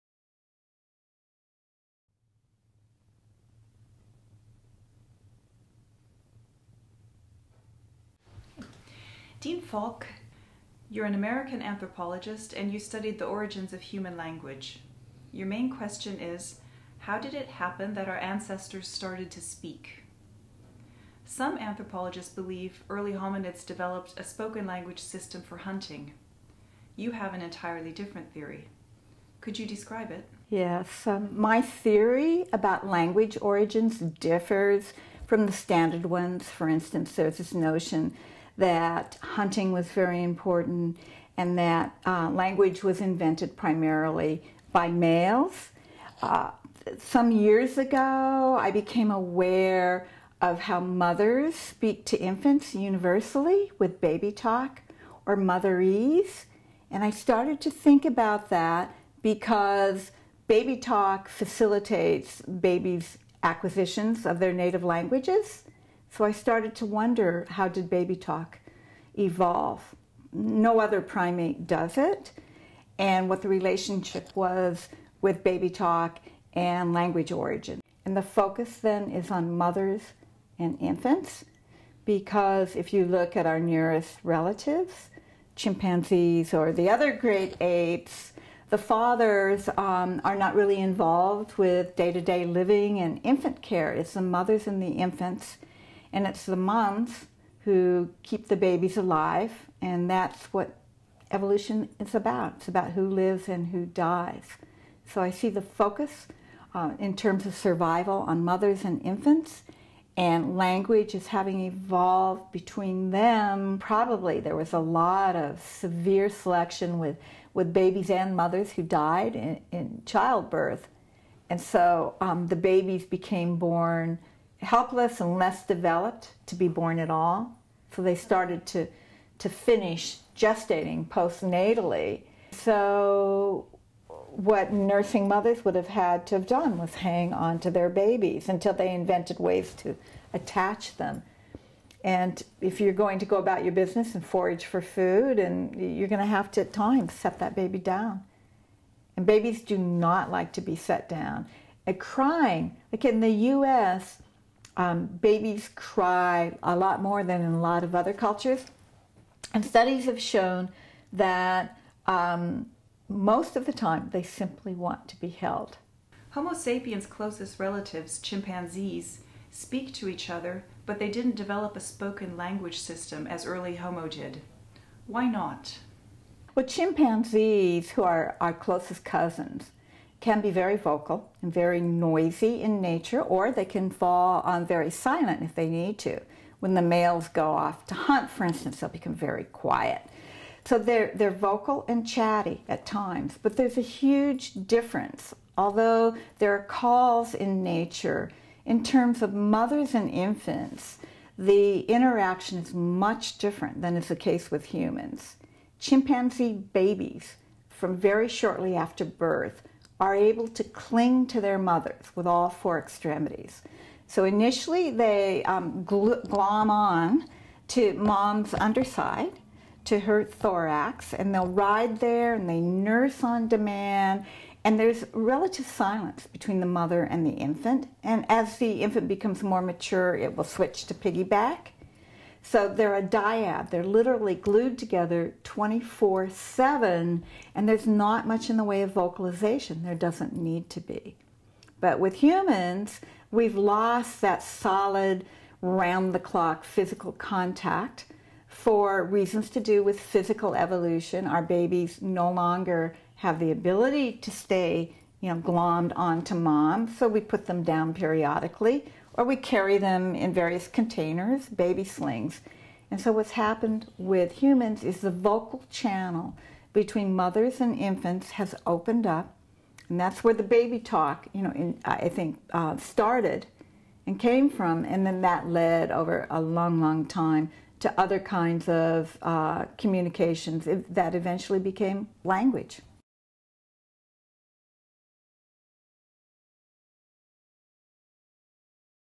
Dean Falk: Wie kam der Mensch zur Sprache?
Die amerikanische Anthropologin Dean Falk sprach an der UZH, wie der Mensch zur Sprache kam. Ihre Theorie in 4 Minuten erfahren Sie